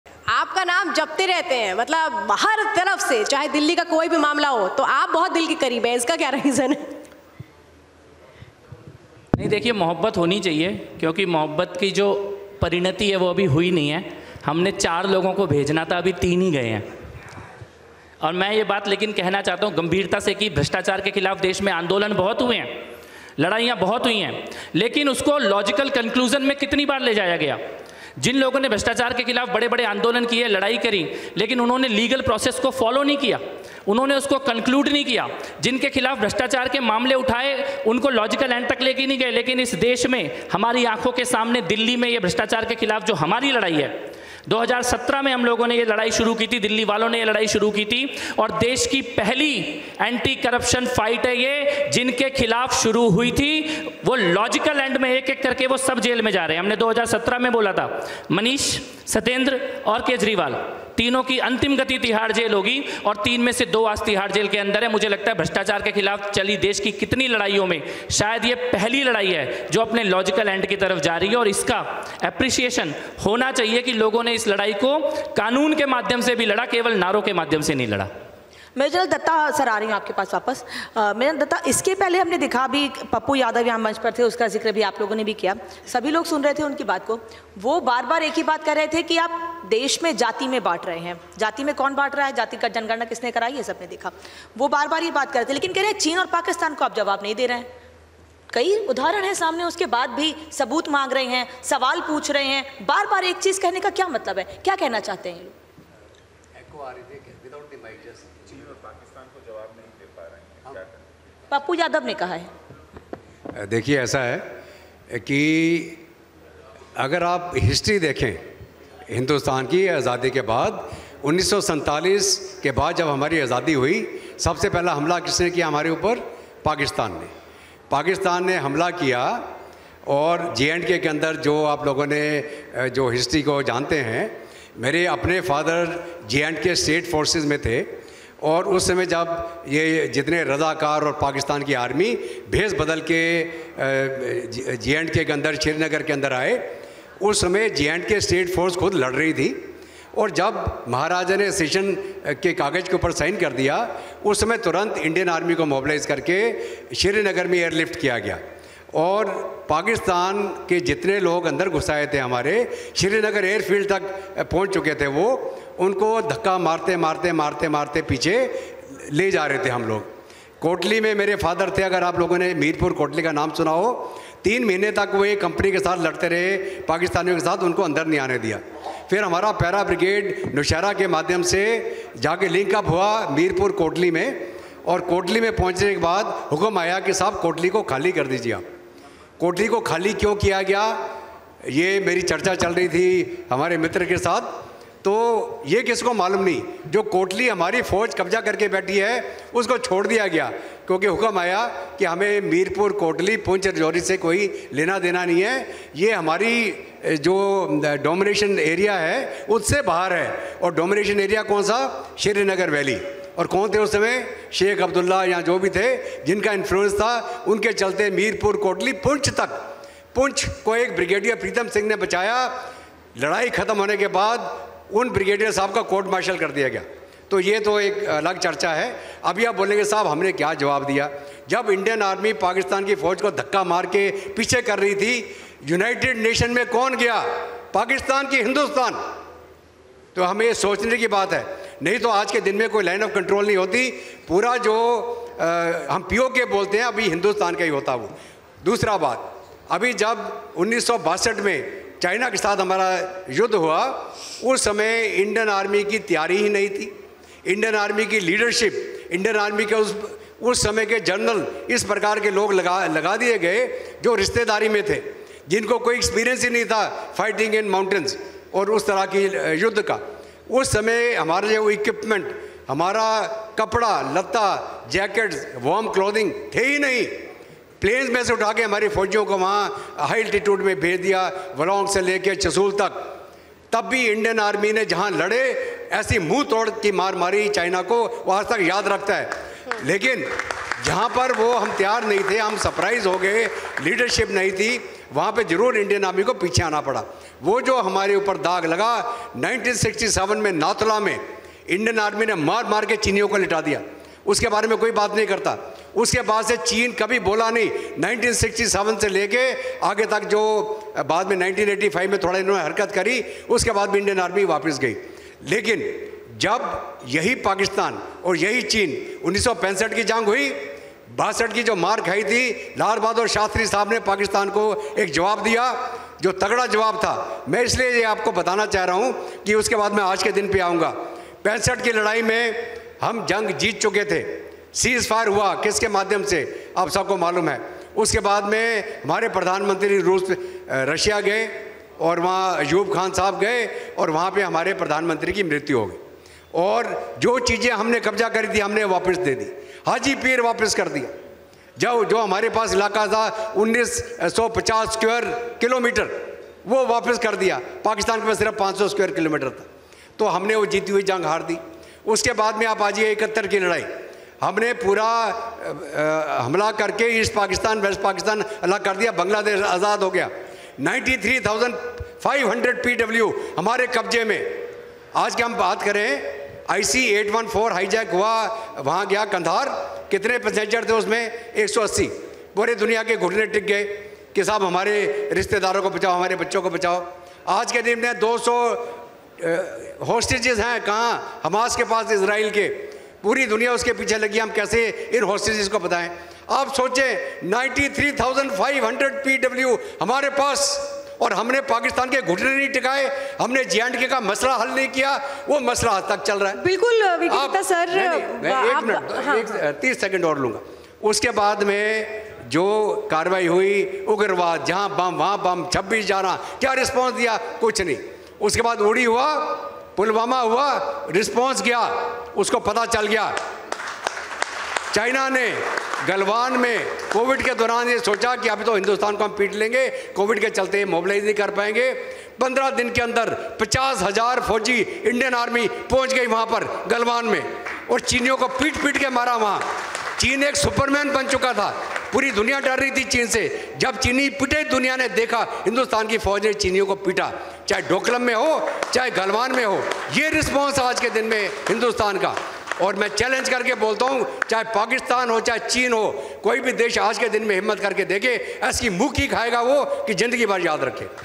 Republic Bharat Summit: Kapil Mishra Slams Opposition Says, 'Corruption Not Eliminated...'
BJP Leader Kapil Mishra speaks on Historic Inauguration of Ram Temple in Ayodhya on January 22.